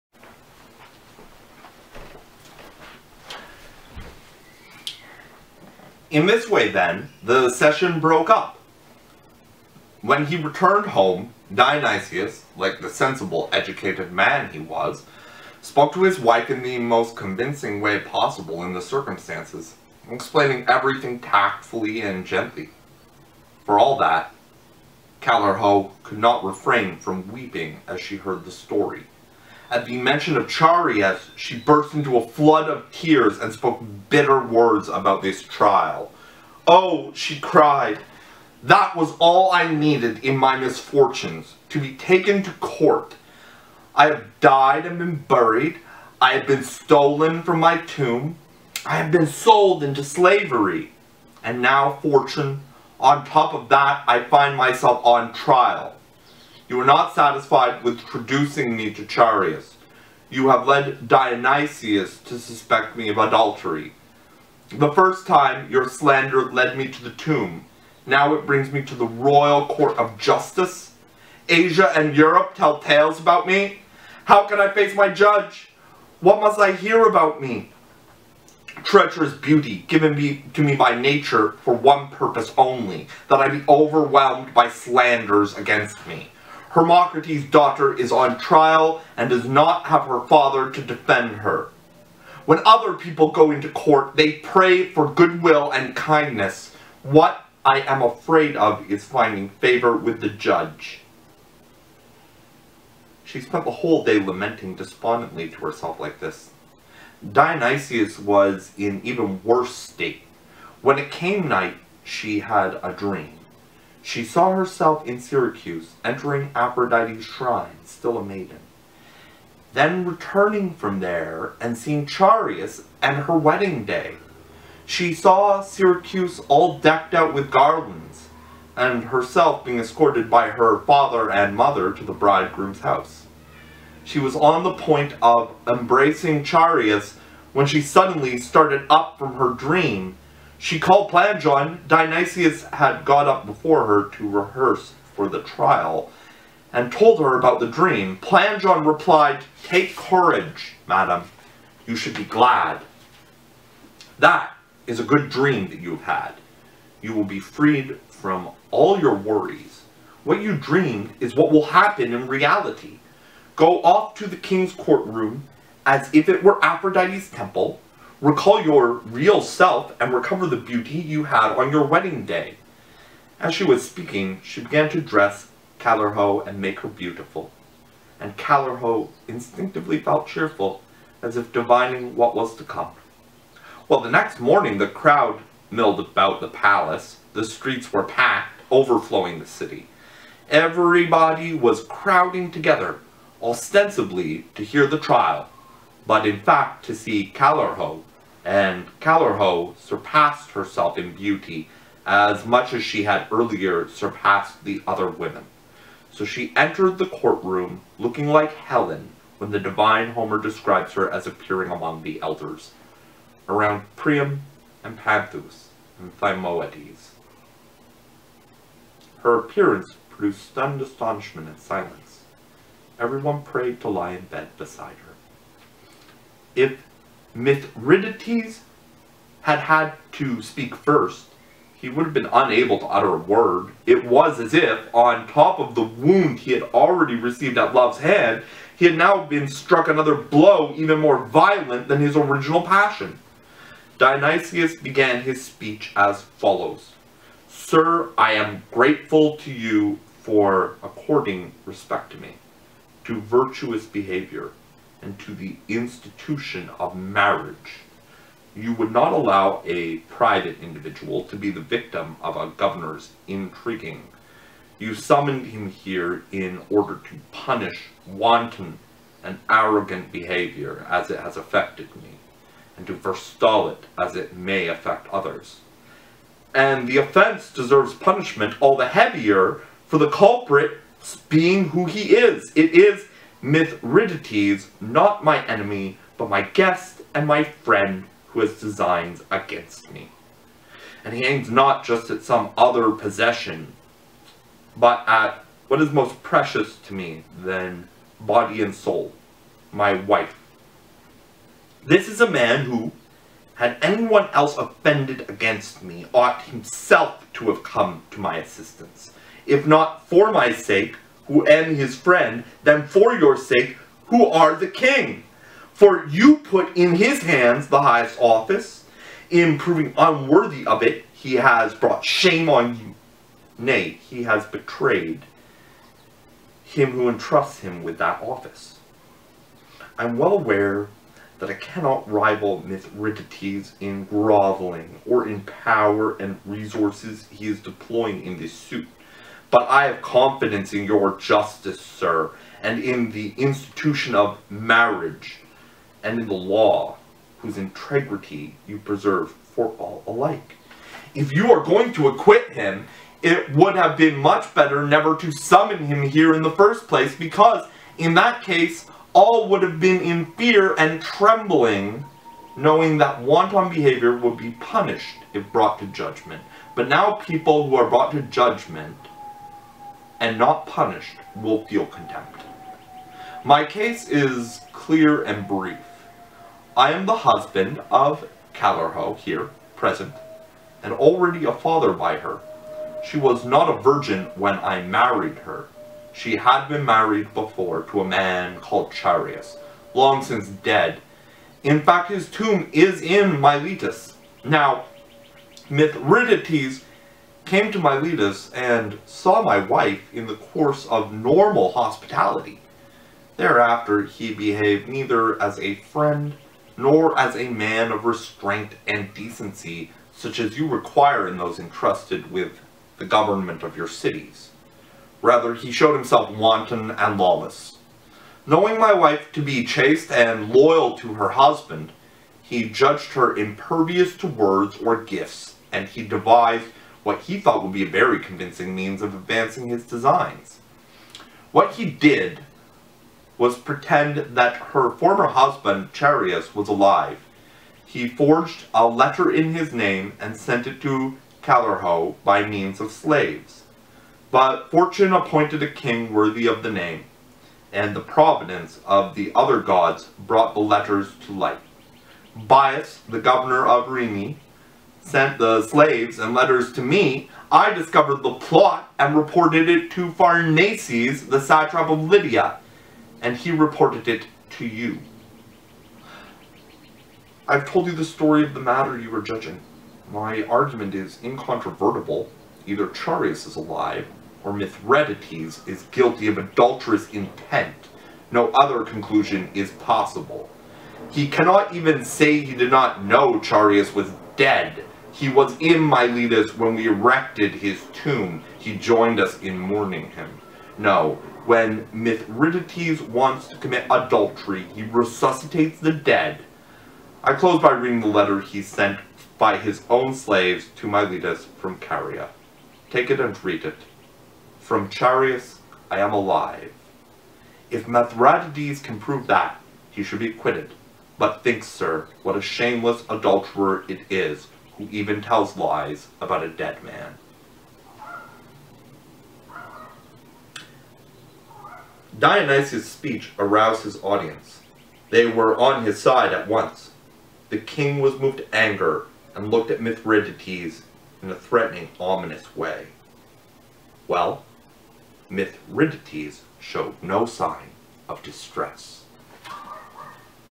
Chaereas and Callirhoe, Book Five, Chapers five and six.
Reardon, B.P. Collected Ancient Greek Novels. Berkeley: University of California Press, 1989.